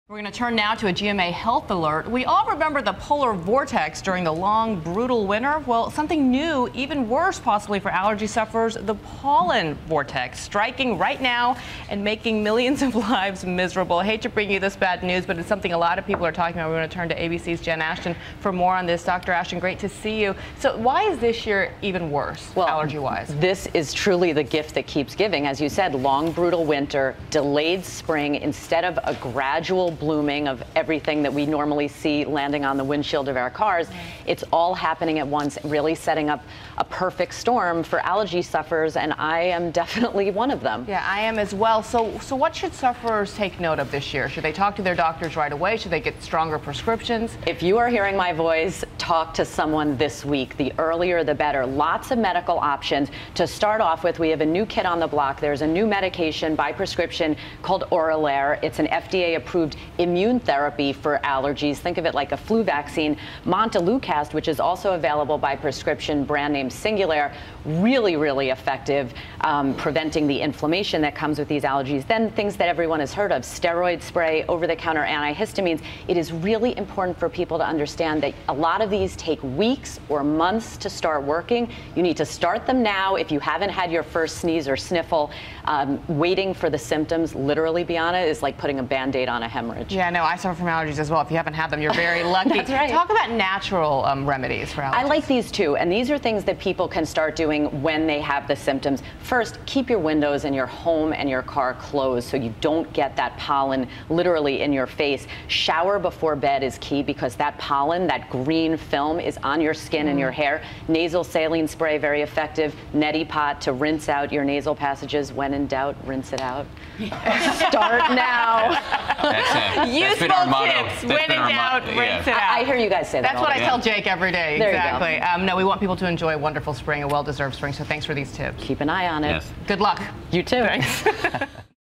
Pollen Vortex: Allergy Season Peaks Early
Long winter and delayed spring are making allergies worse.